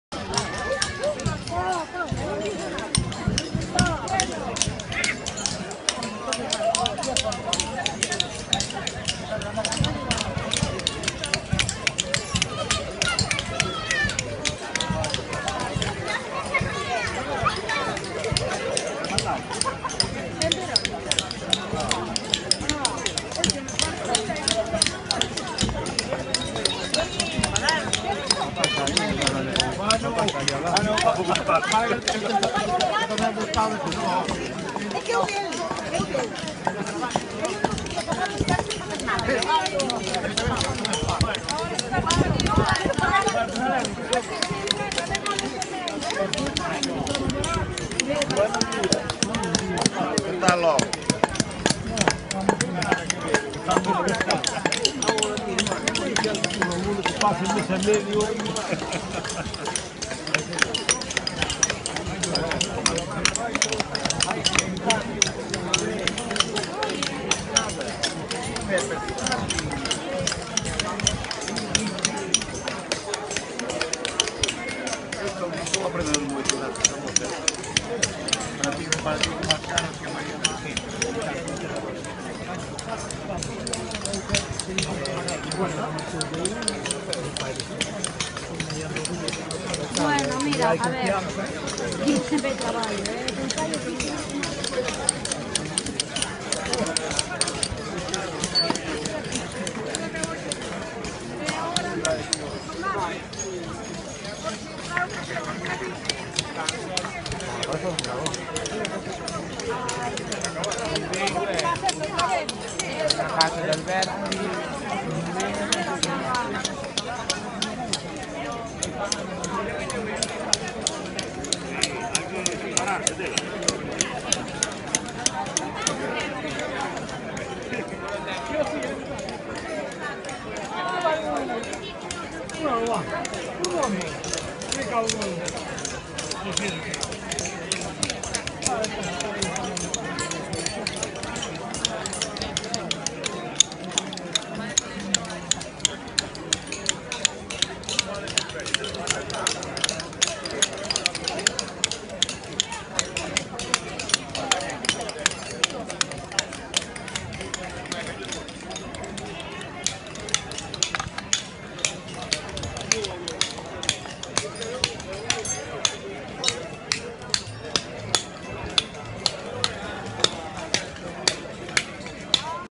Feira da Canteira dos Callos e da Artesanía de Parga
La Feira da Canteira dos Callos e da Artesanía de Parga, en la que se celebró el vigésimo certamen de canteiro. En él participaron 19 artesanos de toda Galicia y se entregaron  tres premios, con un valor total de 1.400 euros.

Se repartieron unas mil raciones de callos y hubo demostraciones en directo de artesanos e otras especialidades. Como novedad hubo pruebas ecuestres y el programa se completó con juegos populares, música y rutas de senderismo y bicicleta. VÍDEO: M.M.